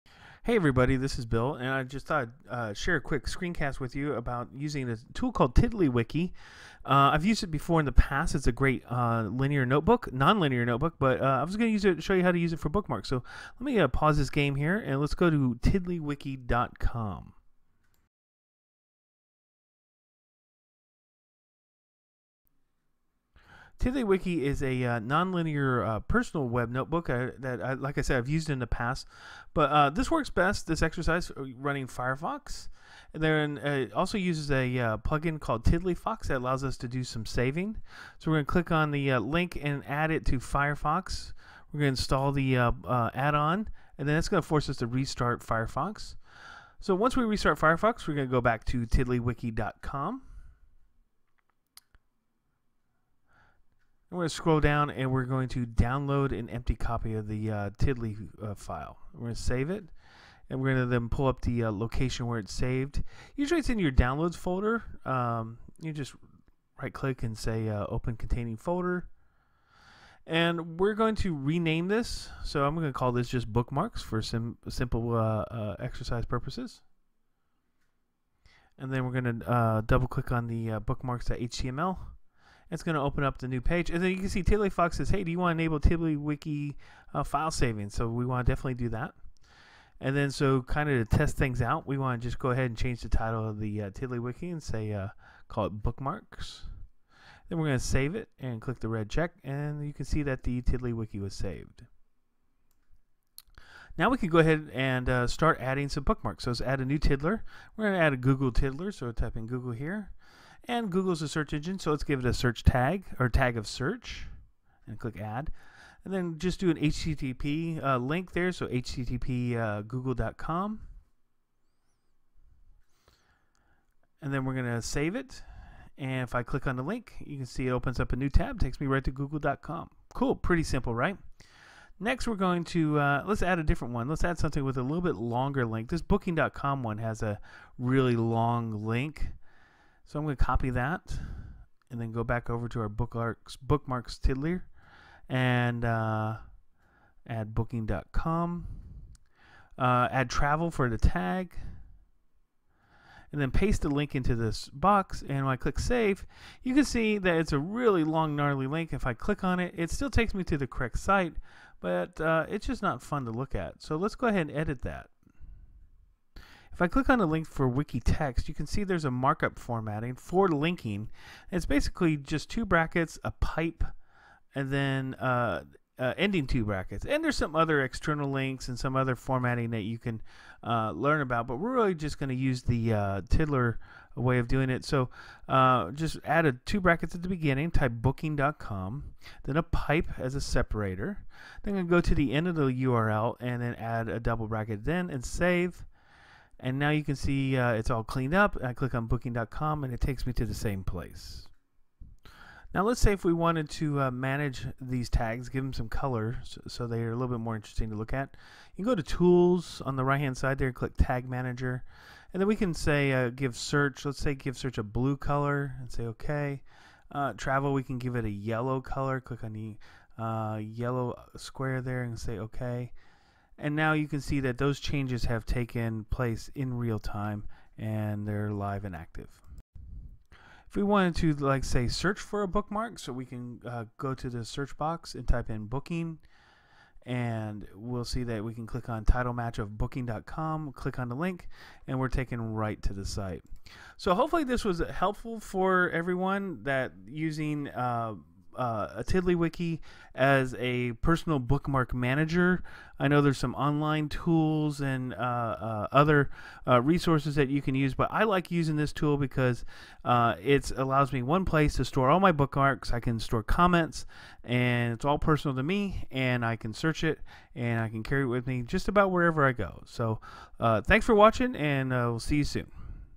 Tiddlywiki Bookmarks
Use Tiddlywiki as your own personal bookmark manager.  It's fast, searchable and you can carry it with you.